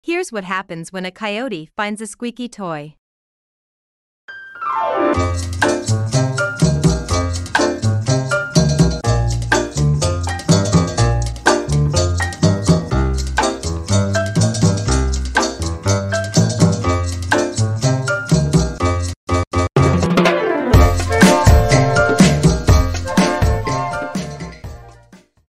So it would seem coyotes love squeaky toys too 🤭
Credits to u/Squanchinthepark on Reddit!